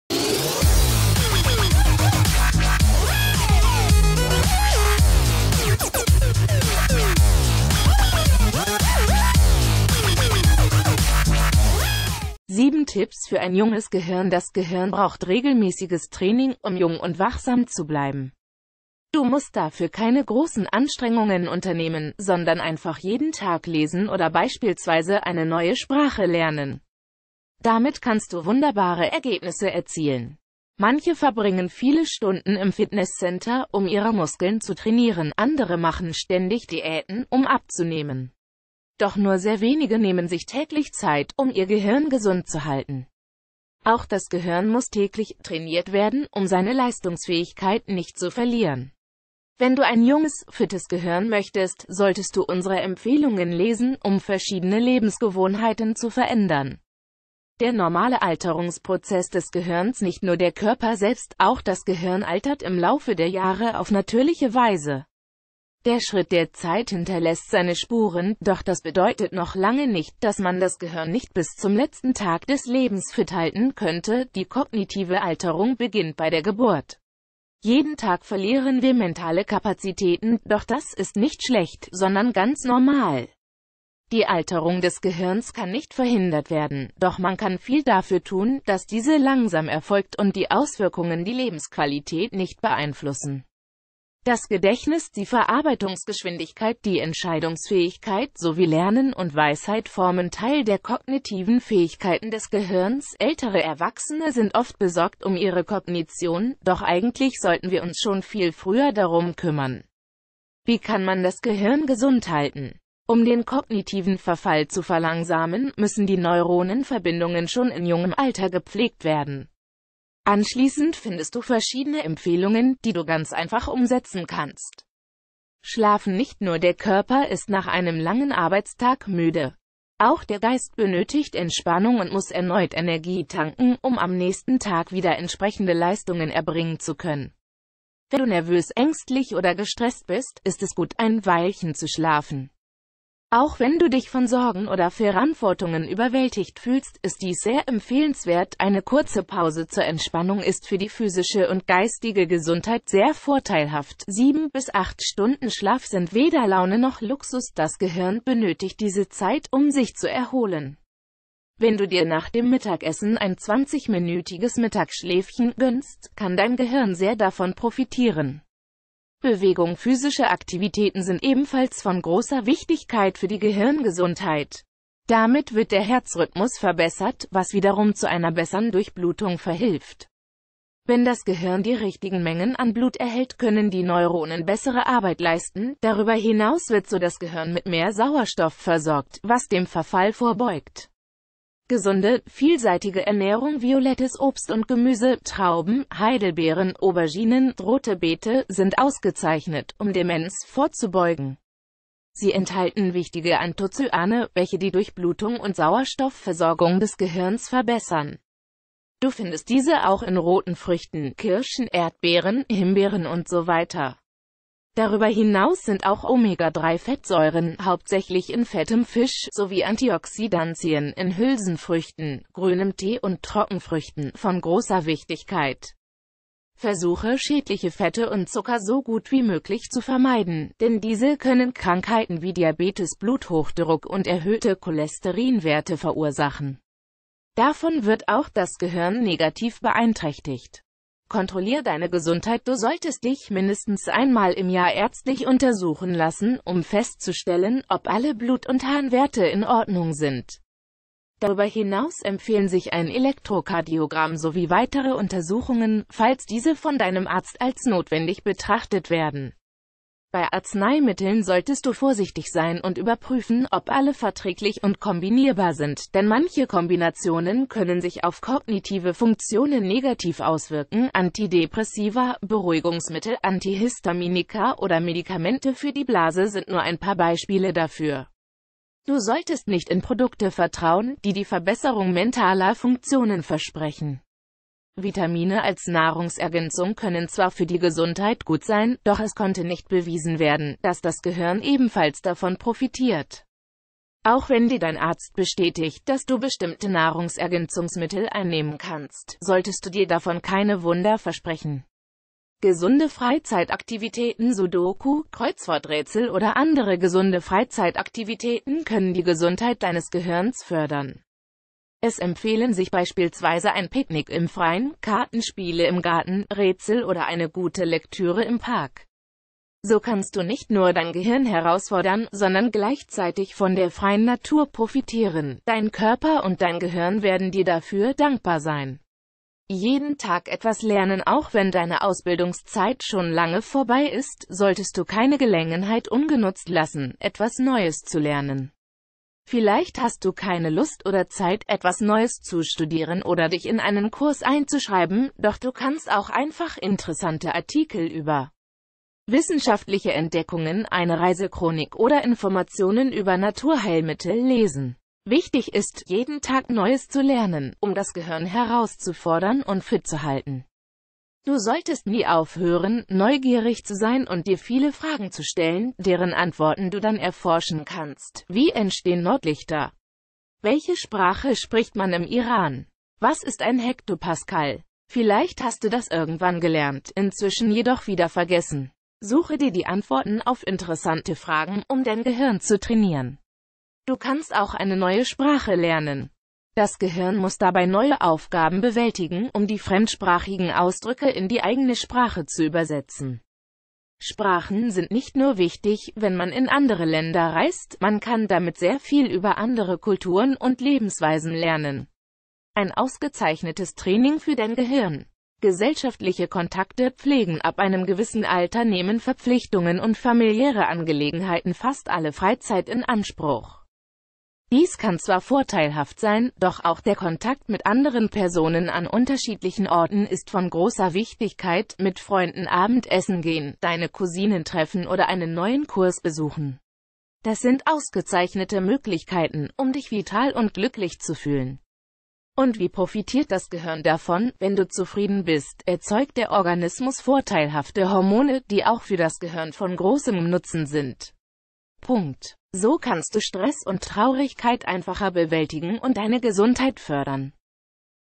7 Tipps für ein junges Gehirn
7 Tipps für ein junges Gehirn
7 Tipps für ein junges Gehirn Das Gehirn braucht regelmäßiges Training, um jung und wachsam zu bleiben. Du musst dafür keine großen Anstrengungen unternehmen, sondern einfach jeden Tag lesen oder beis...